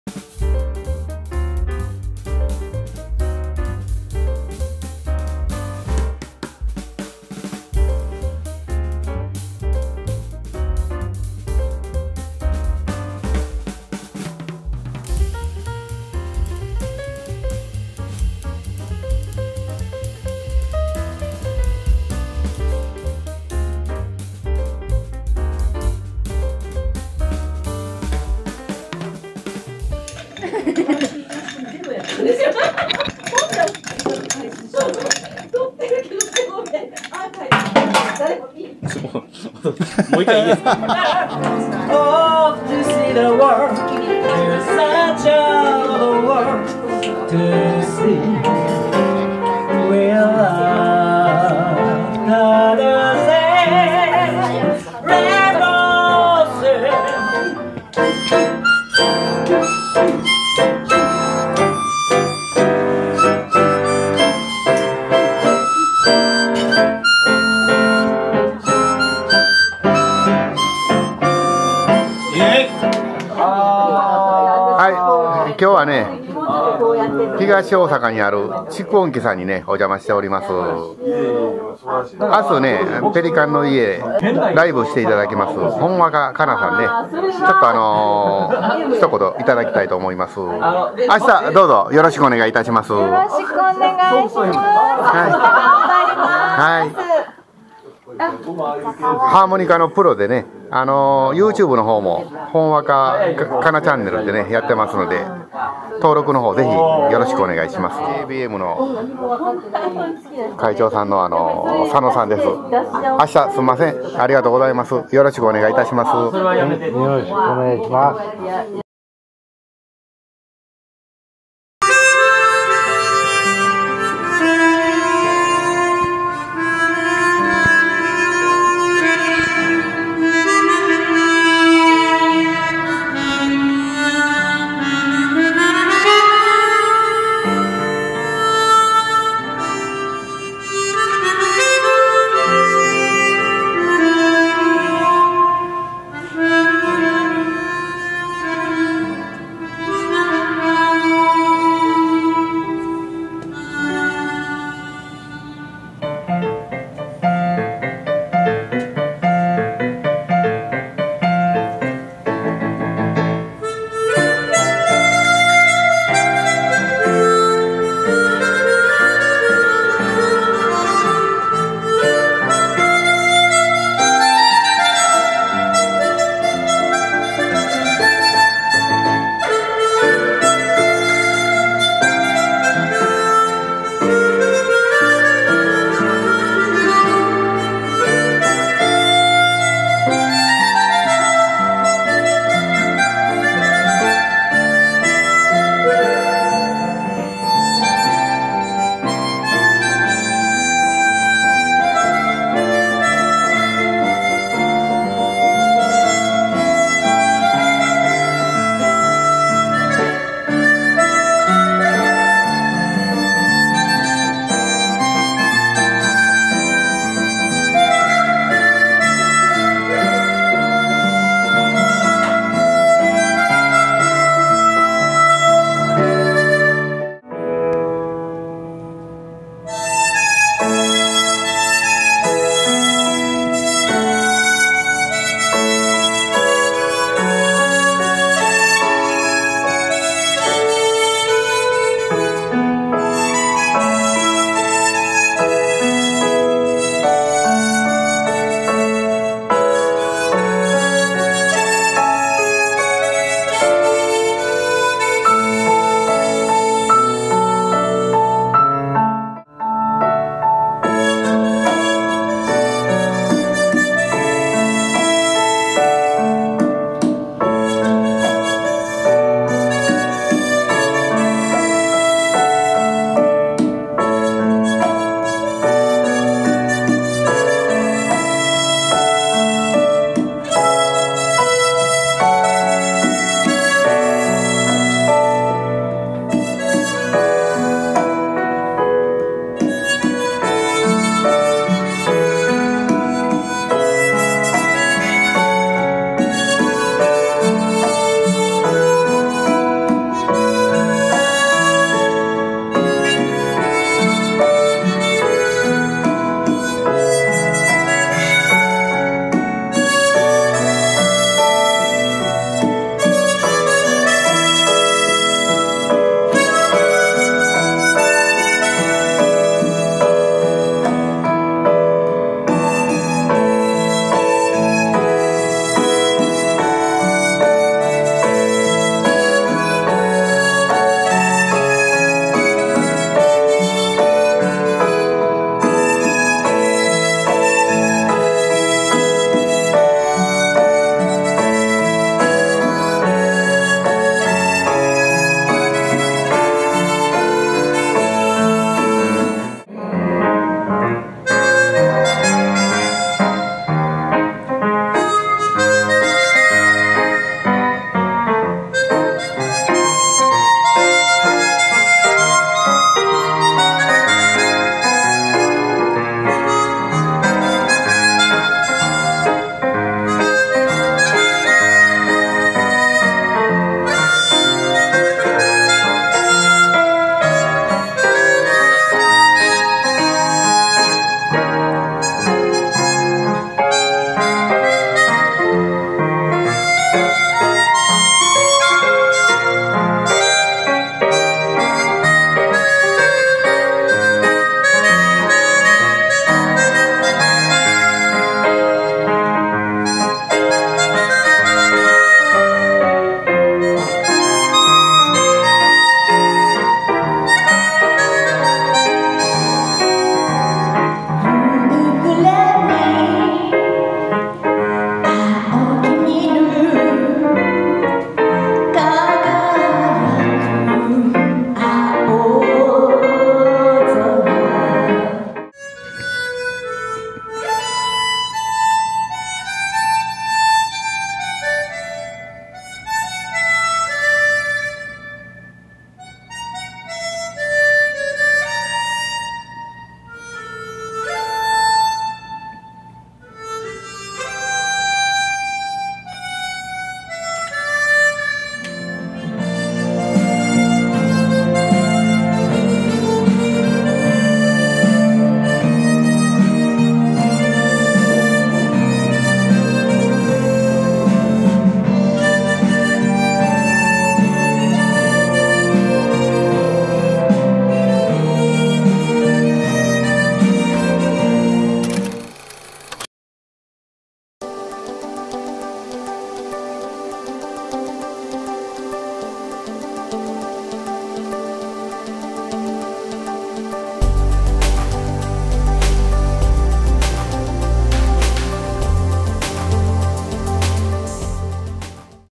ほんわかかなさん演奏 癒しのライブ
喫茶ペリカンの家で行われた、ほんわかかなさんによるハーモニカライブの様子です。癒されます。喫茶ペリカンの家